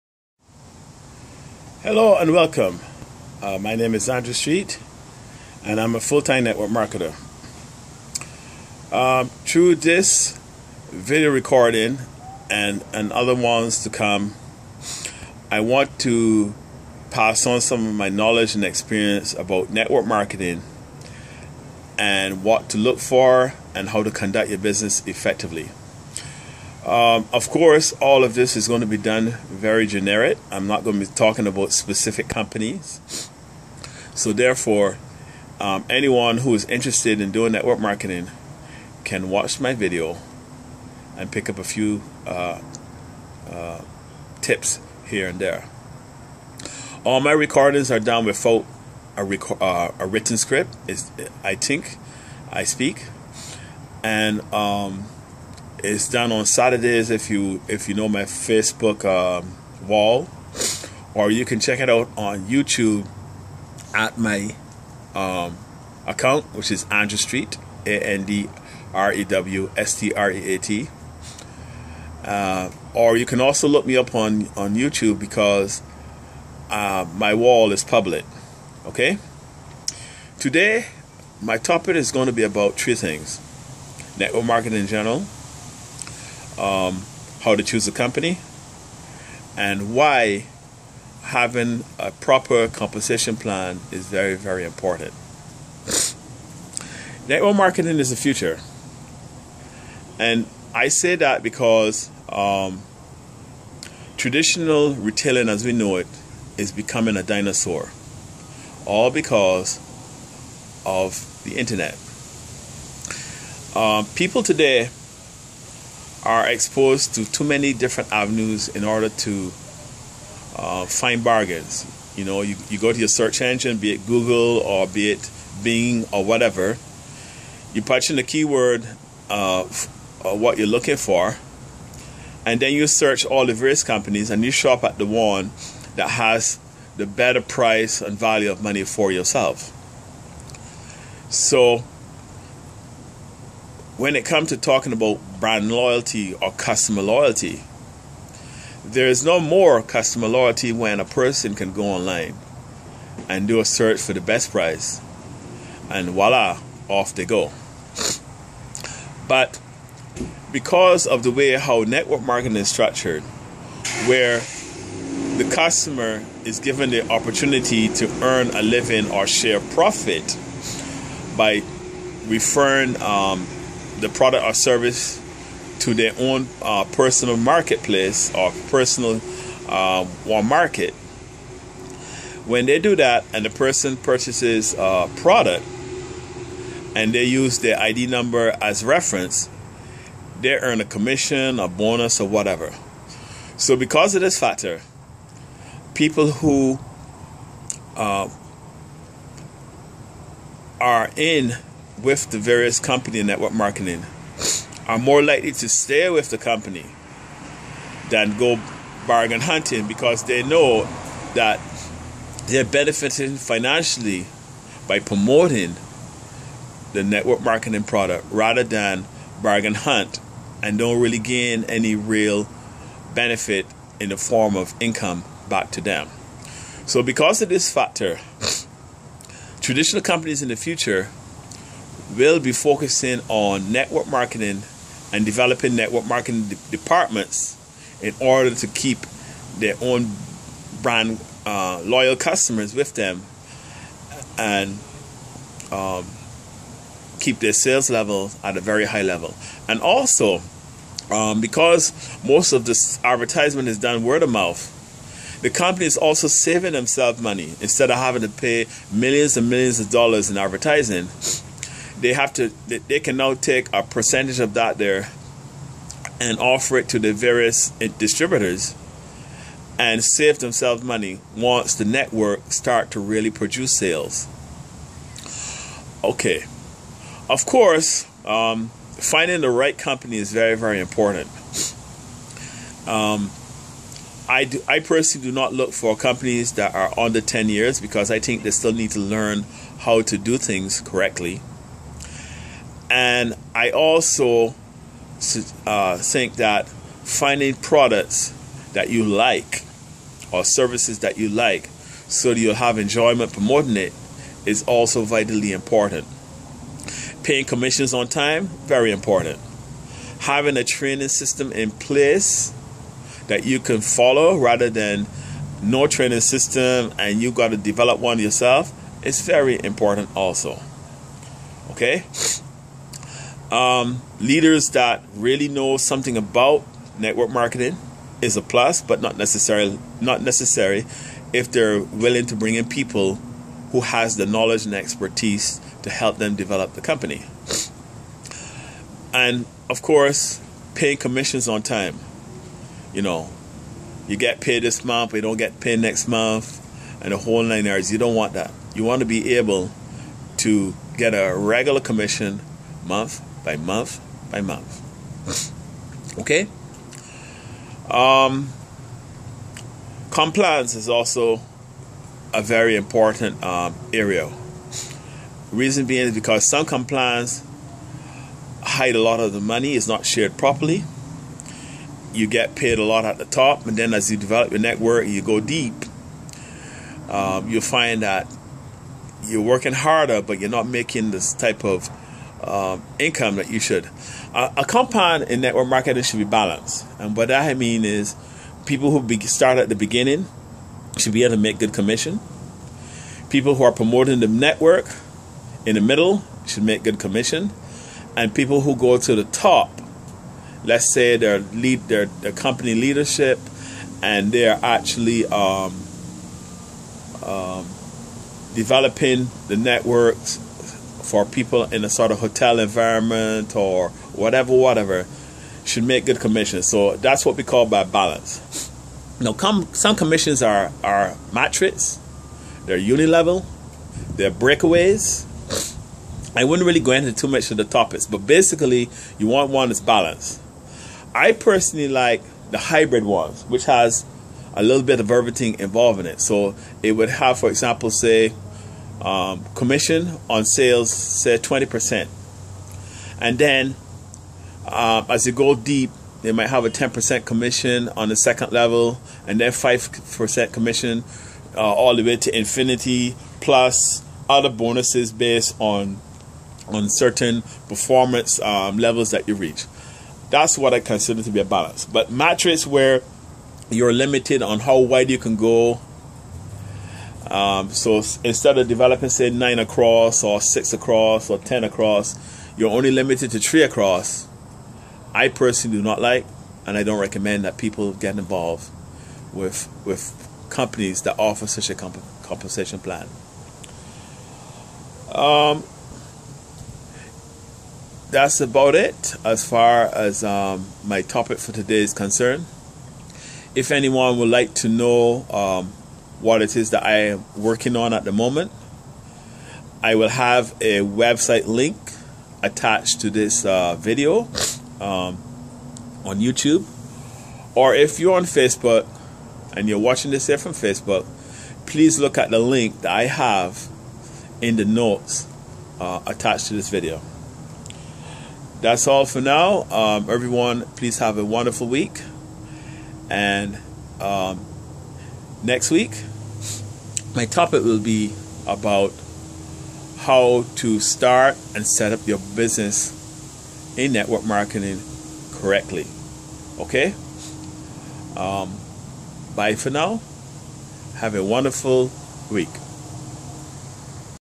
Network Marketing - Why it is important in the future
Andrew Streat explaining his reasons about network marketing and its importance in the future.